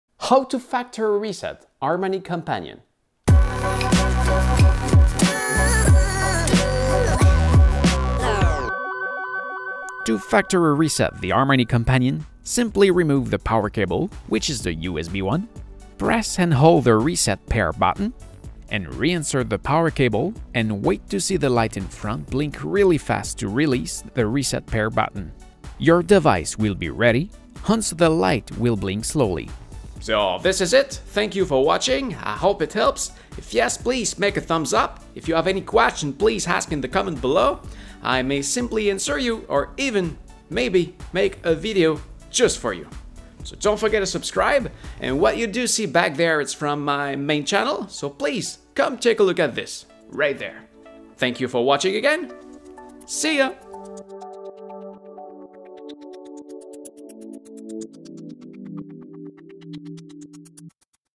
How to Factory Reset Harmony Companion
Do you have a problem with your harmony remote?
Or maybe you only try to sell it and want to set it back to its factory settings.
Let me show you how to do it.